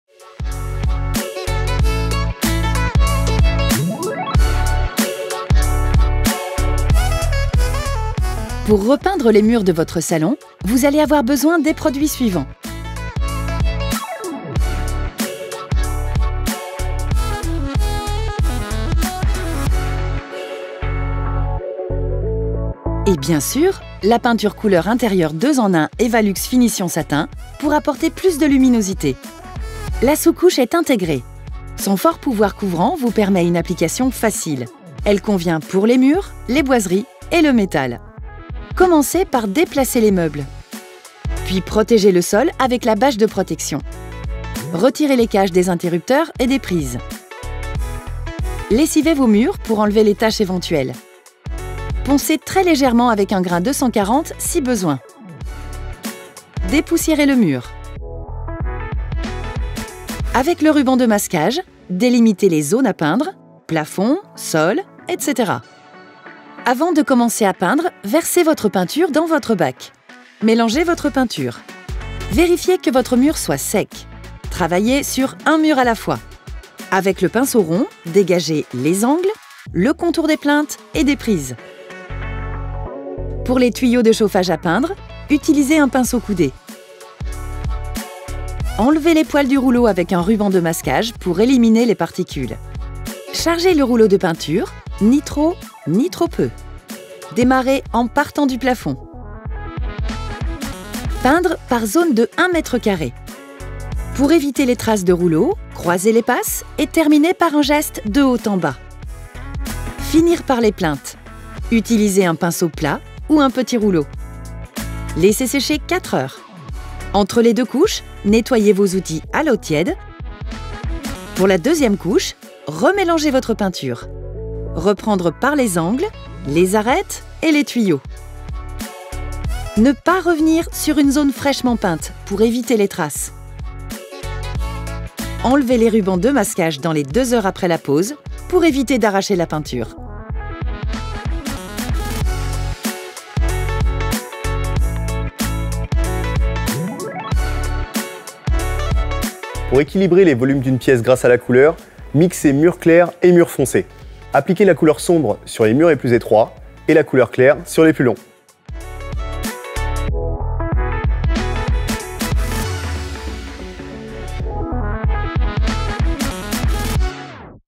Comment peindre les murs de votre salon ?
Découvrez notre tutoriel vidéo réalisé pour Evalux : apprenez comment peindre les murs de votre salon

*Préparer les murs du salon*

1- Si possible, enlevez tous les meubles du salon, ainsi que toutes les décorations murales (cadres, miroirs, barres à rideaux...). Si vous ne pouvez pas retirer les plus volumineux, regroupez-les au centre de la pièce et recouvrez-les d’une bâche ou de vieux draps le temps des travaux.
2- Protégez le sol du salon  avec une bâche plastique que vous fixez au ras du mur avec du ruban de masquage.
3- Coupez l’électricité et retirez les caches des interrupteurs et des prises de courant, ainsi que les luminaires ou les appliques.
4- Lessivez les murs de votre pièce à l’éponge et à l’eau claire s’ils sont peu sales ou avec un produit dégraissant type lessive Saint-Marc.
5- Poncez légèrement les murs avec un abrasif fin (grain 240) : cela facilitera l’accroche de la peinture et améliorera les finitions.
6- Dépoussiérez soigneusement les murs du salon avec une brosse douce ou un chiffon légèrement humide.

*Appliquer la peinture sur les murs de votre salon*

1- Versez la peinture dans un bac à peinture et donnez à la teinte un aspect bien homogène à l’aide d’un mélangeur.
2- Travaillez sur un mur du salon à la fois et commencez par le haut du mur. Dégagez les angles en peignant avec un pinceau rond ou à rechampir. Peignez aussi les contours des prises ou interrupteurs.
3- Utilisez un pinceau coudé pour atteindre le mur derrière les tuyaux de chauffage ou les radiateurs.
4- Avant d’utiliser votre rouleau, enlevez les poils  pour éviter qu’ils se collent sur votre mur, en appliquant dessus un ruban de masquage que vous enlevez ensuite. Plongez le rouleau dans le bac à peinture et essorer-le contre la grille pour enlever l’excédent.
5- Peindre chaque mur du salon en commençant toujours par le haut, en croisant les passages (une fois à l’horizontale, une fois à la verticale). Peignez par zone de 1 m² pour une application bien homogène et terminez toujours par un geste de haut en bas pour lisser la peinture et avoir de belles finitions sur les teintes.
6- Terminez par les plinthes si vous les peignez de la même couleur, en utilisant un petit rouleau ou un pinceau large. Laissez sécher au moins 4h en rinçant vos outils entre les deux couches.
7- Appliquez la deuxième couche de la même façon, après avoir mélangé la peinture. Laissez sécher et retirez les rubans de masquage dans les 2h en conservant un angle de 45° pour ne pas abîmer la peinture. Pour finir vos travaux, ôter les bâches et attendez 24h avant de réinstaller les meubles.

Sur Instagram : bricolo.factory
Sur Linkedin : Bricolo Factory